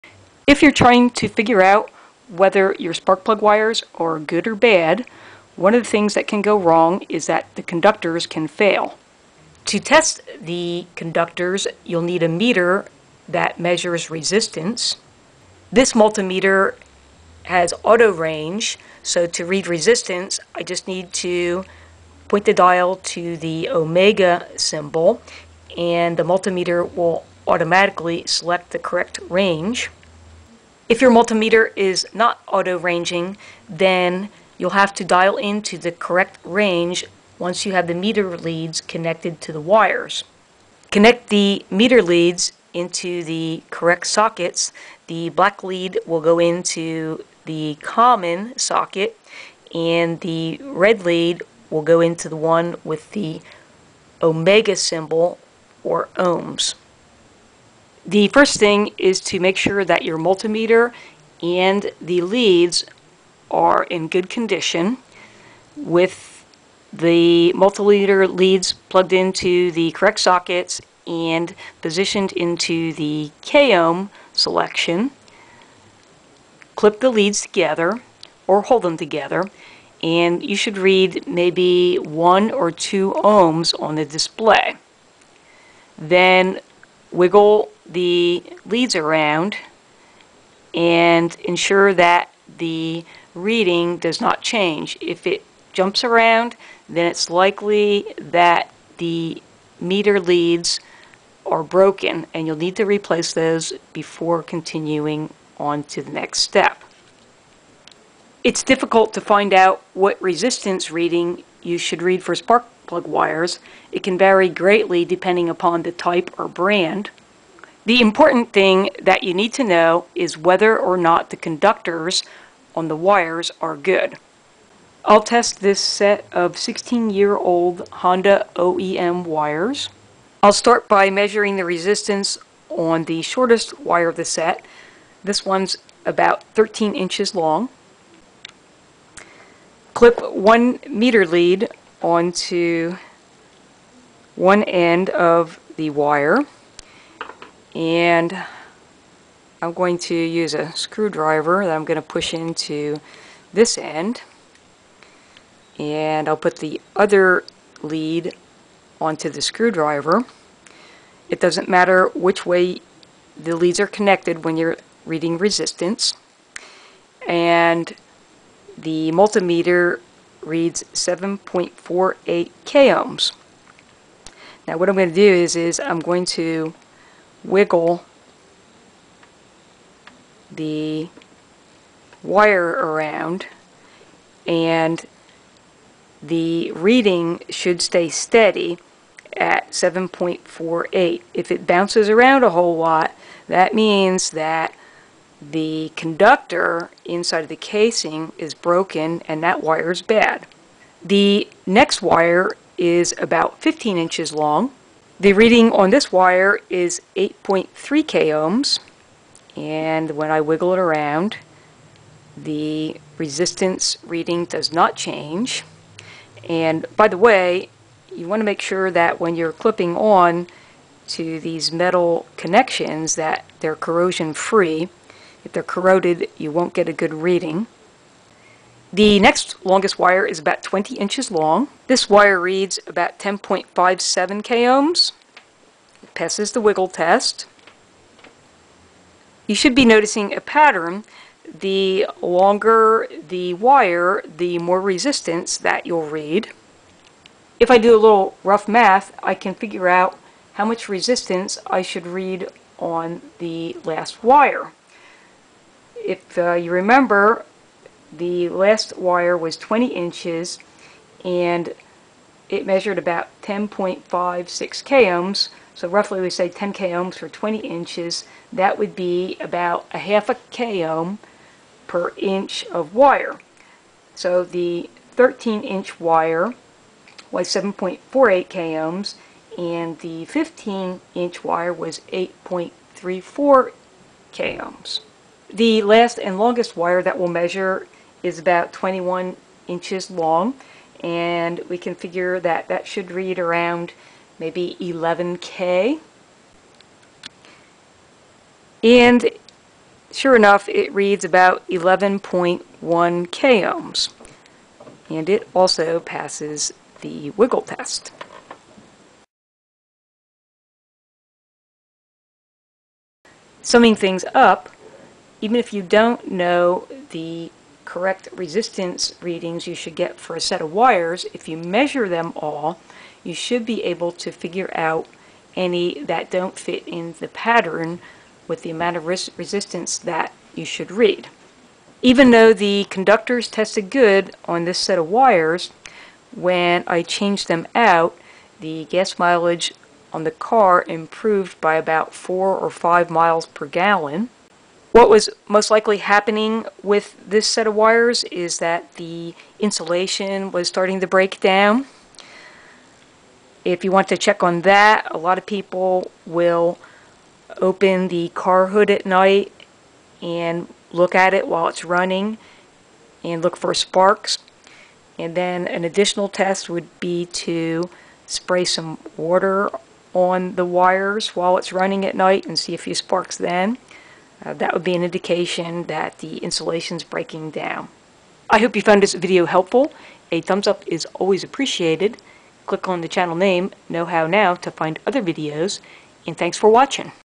Test Spark Plug Wires With Multimeter
No resistance specs or chart is needed to figure out the correct spark plug wire resistance on any set of spark plug wires.   A spark plug wire test with a multi meter can determine if the conductors are good.